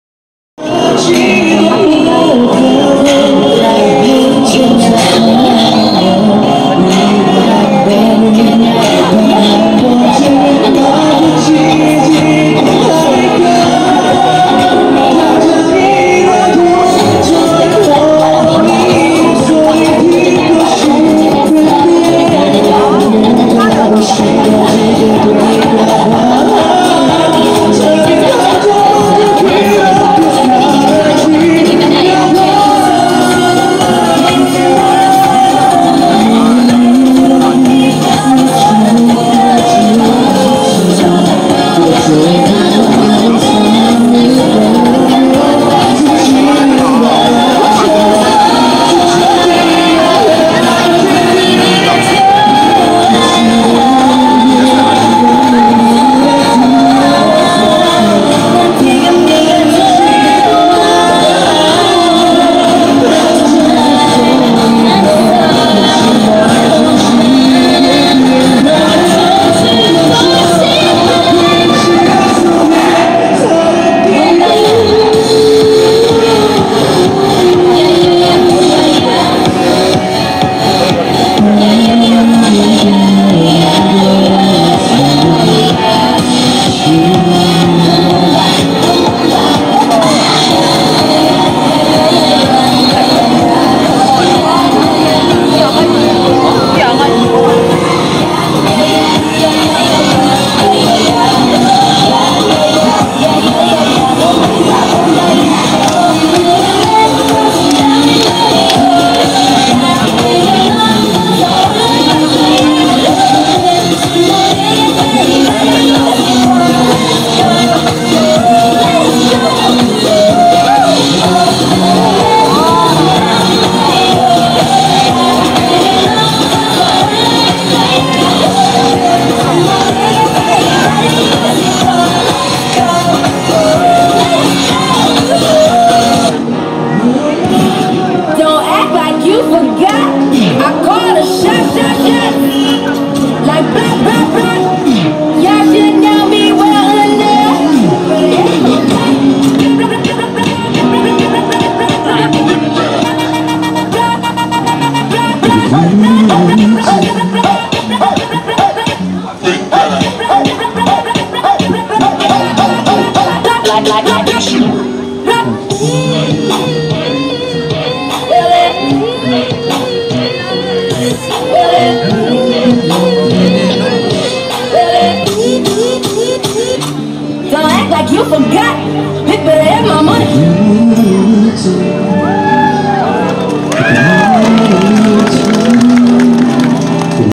Red Spark performing at Hongdae District Seoul Video Test using LG Stylus 2.
Kpop Group performing at Hongdae District using LG Stylus 2 with Osmo mobile 2 gimbal.